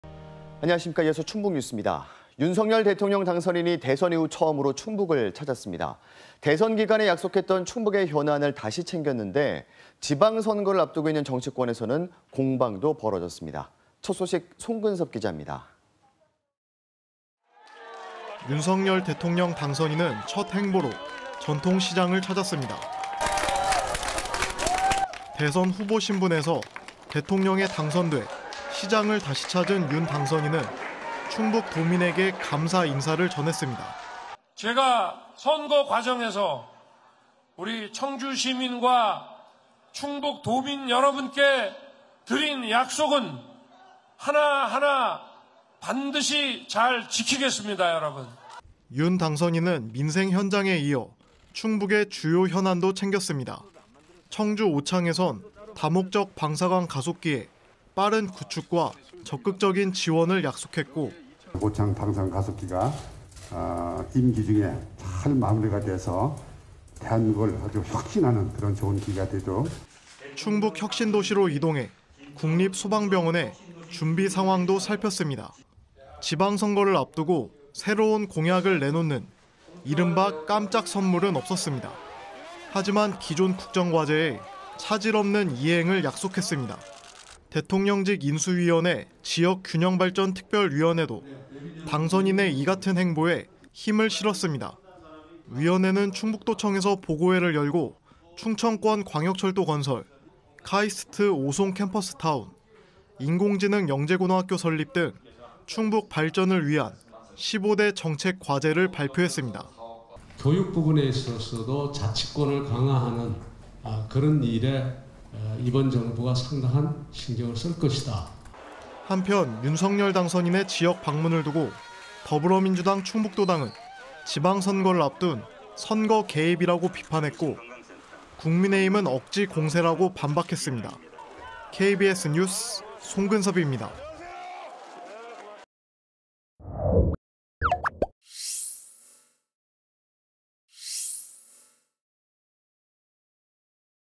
충북 현안 챙긴 윤석열 당선인…선거 앞두고 공방도 KBS 20220429
[앵커]

 윤석열 대통령 당선인이 대선 이후 처음으로 충북을 방문했습니다.

 대선 기간 약속했던 충북 현안을 다시 챙겼는데, 지방선거를 앞둔 정치권에선 공방도 벌어졌습니다.

 보도에 송근섭 기자입니다.

 [리포트]

 윤석열 대통령 당선인은 첫 행보로 전통시장을 찾았습니다.

 ["와아아~!"]

 대선 후보 신분에서, 대통령에 당선돼 시장을 다시 찾은 윤 당선인은 충북도민에게 감사 인사를 전했습니다.

 [윤석열/대통령 당선인 :  "제가 선거 과정에서 우리 청주시민과 충북 도민 여러분께 드린 약속은 하나하나 반드시 잘 지키겠습니다. 여러분."]

 윤 당선인은 민생 현장에 이어 충북의 주요 현안도 챙겼습니다.

 청주 오창에선 다목적 방사광 가속기의 빠른 구축과 적극적인 지원을 약속했고,

 [이시종/지사  :  "오창 방사광 가속기가 (대통령) 임기 중에 잘 마무리돼서, 대한민국을 아주 혁신하는 계기가 되도록."]

 충북 혁신도시로 이동해 국립 소방병원의 준비 상황도 살폈습니다.

 지방선거를 앞두고 새로운 공약을 내놓는 이른바 '깜짝 선물'은 없었습니다.

 하지만, 기존 국정과제의 차질 없는 이행을 약속했습니다.

 대통령직 인수위원회 지역 균형발전 특별위원회도 당선인의 이 같은 행보에 힘을 실었습니다.

 위원회는 충북도청에서 보고회를 열고 충청권 광역철도 건설, 카이스트 오송캠퍼스 타운, 인공지능 영재고등학교 설립 등 충북 발전을 위한 15대 정책 과제를 발표했습니다.

 [김병준/대통령직 인수위원회 지역균형발전특별위원장  :  "(영재고 등) 교육 부분에 있어서도 자치권을 강화하는 그런 일에 이번 정부가 상당한 신경을 쓸 것이다."]

 한편 윤석열 당선인의 지역 방문을 두고 더불어민주당 충북도당은 지방선거를 앞둔 '선거 개입'이라고 비판했고  국민의힘은 '억지 공세'라고 반박했습니다.

 KBS 뉴스 송근섭입니다.

 촬영기자:박준규